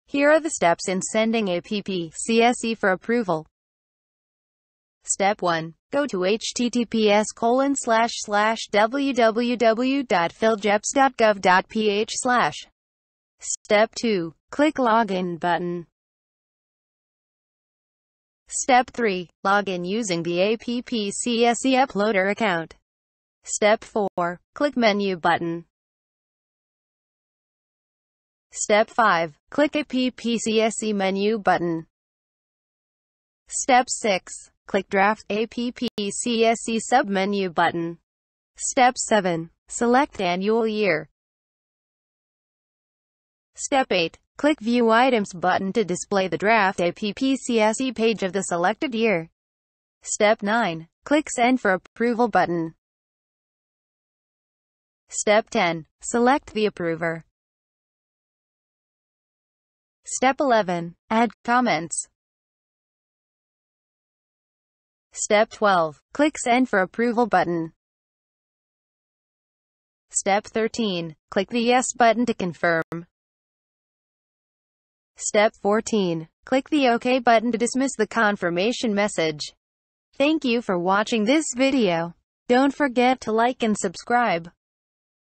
Send APP CSE for Approval
This is a video tutorial of how to Send APP-CSE for Approval